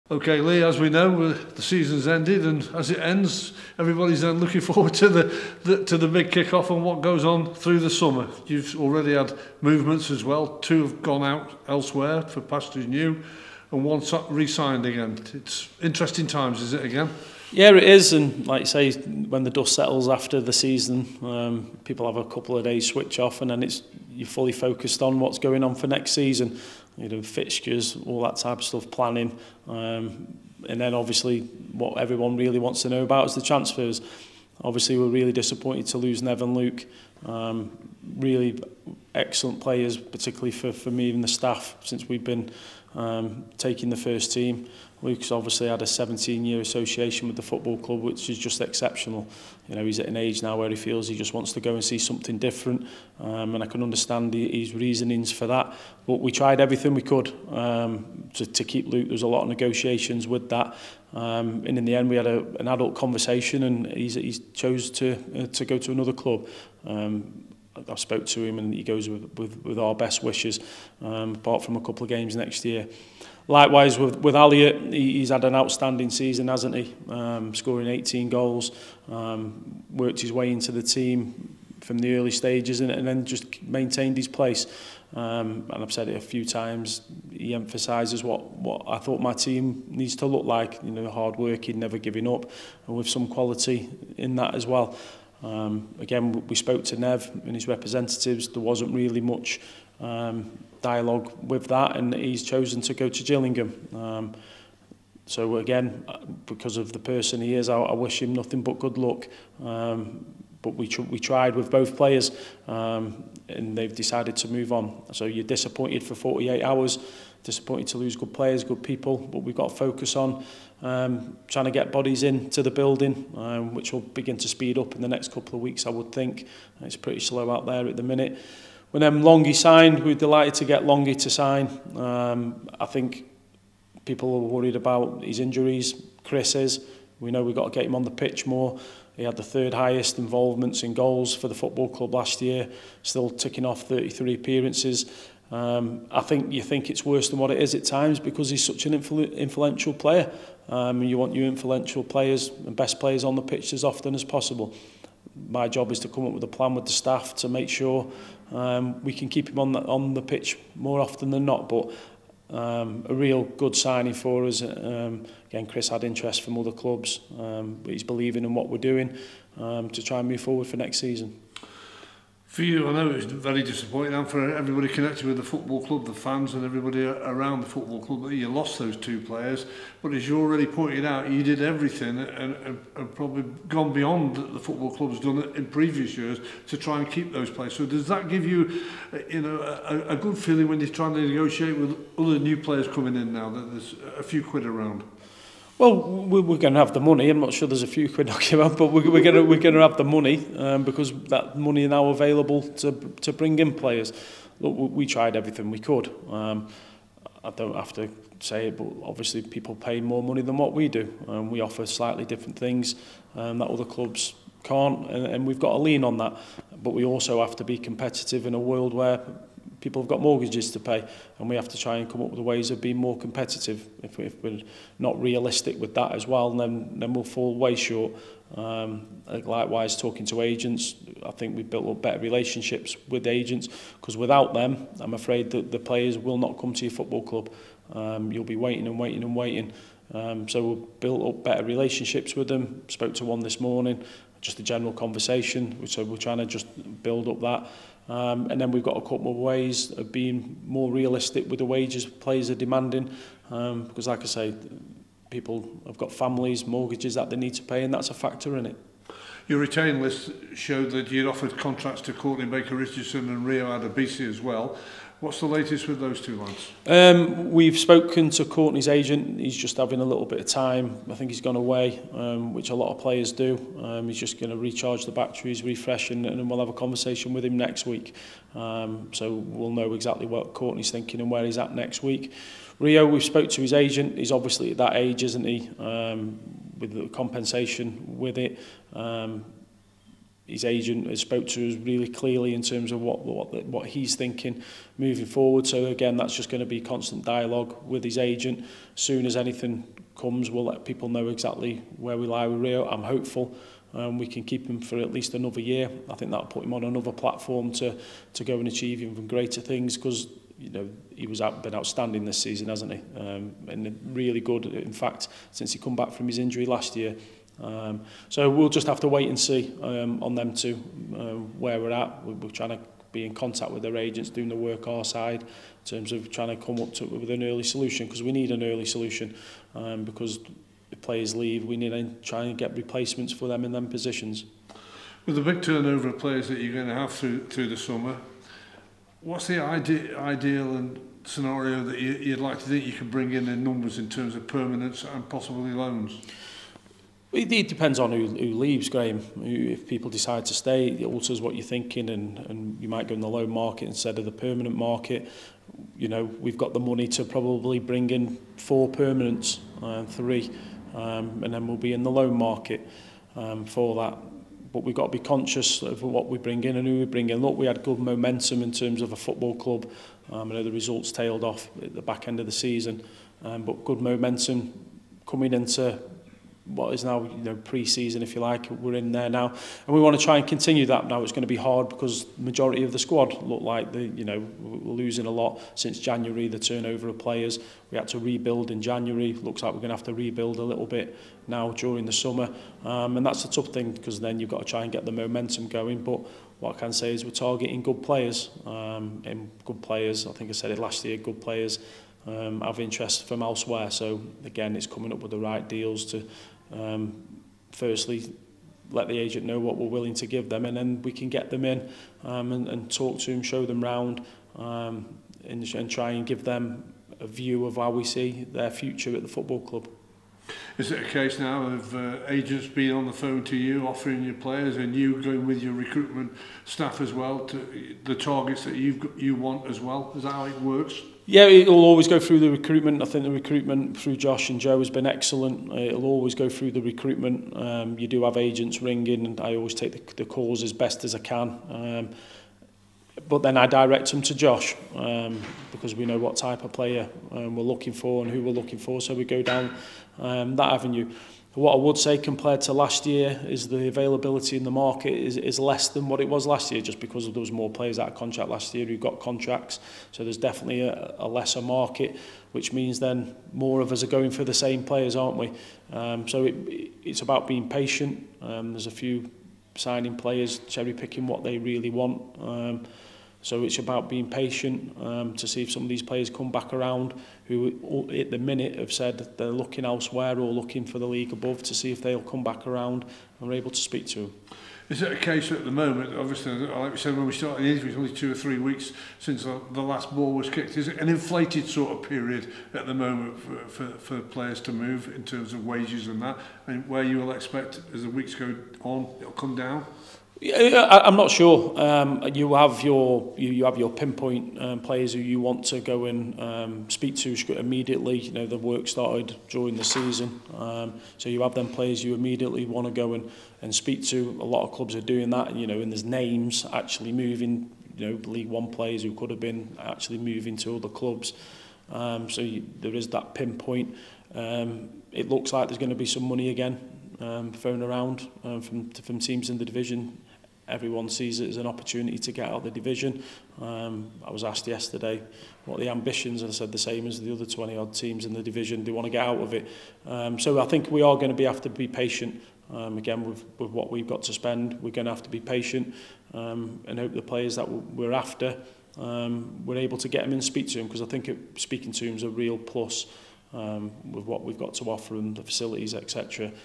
END OF SEASON UPDATE | The Boss
The boss sat down to reflect on the 2023/24 season, his transfer plans and the departures of Elliott Nevitt and Luke Offord.